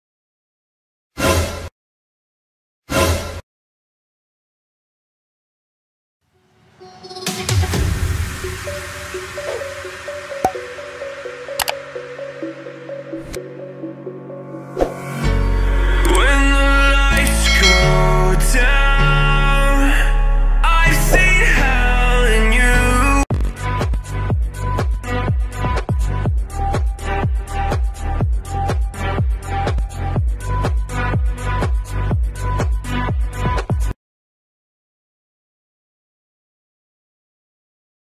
Ewolucja loga Roblox Studio (2006-2022)
Opis
Cześć! Witajcie w serii ewolucja na moim kanale! Dziś wam pokaże logo roblox studio jak się zmieniało na przestrzeni lat. Jeśli masz pomysł na kolejny odcinek z tej serii to napisz w komentarzu! Zachęcam do subskrypcji kanału! Zapraszam do oglądania!

Reżyseria: Roblox
Scenariusz: Życie
Montaż: Dawid SK3BA TV
Producent: Rockstar